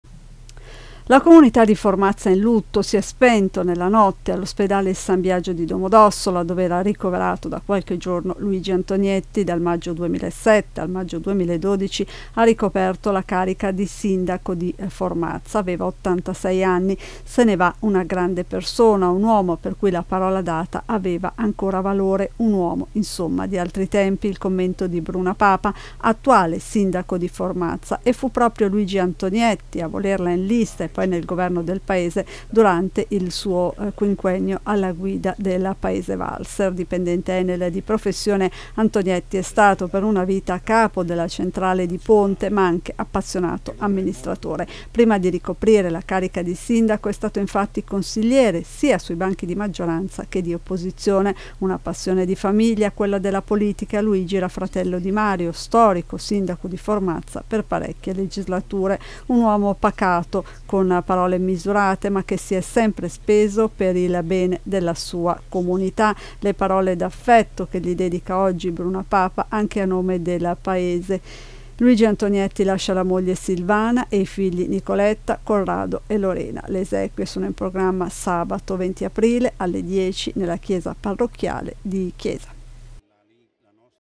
Formazza in lutto per la scomparsa dell’ex sindaco Antonietti
La comunità di Formazza è in lutto. Si è spento nella notte all’ospedale San Biagio dov’era ricoverato da qualche giorno Luigi Antonietti. Dal maggio 2007 al maggio 2012 ha ricoperto la carica di sindaco di Formazza. “Se ne va una grande persona. Un uomo per cui la parola data aveva valore. Un uomo di altri tempi” il primo commento di Bruna Papa, attuale sindaco di Formazza. Fu proprio Antonietti a volerla in lista e poi nel governo del paese durante il suo quinquennio alla guida del paese walser.
Dipendente Enel di professione è stato per una vita capo della centrale di Ponte ma anche appassionato amministratore. Prima di ricoprire la carica di sindaco è stato consigliere, sia di maggioranza che di opposizione. Una passione di famiglia quella della politica. Luigi era fratello di Mario, storico sindaco di Formazza per parecchie legislature. “Un uomo pacato. Con parole misurate ma che si è sempre speso per il bene della sua comunità” le parole d’affetto di Bruna Papa che a nome del paese ha parlato di una perdita che lascerà il segno.
Luig Antonietti lascia la moglie Silvana e i figli Nicoletta, Corrado e Lorena.
Le esequie sono in programma sabato 20 aprile alle 10 nella chiesa parrocchiale di Chiesa.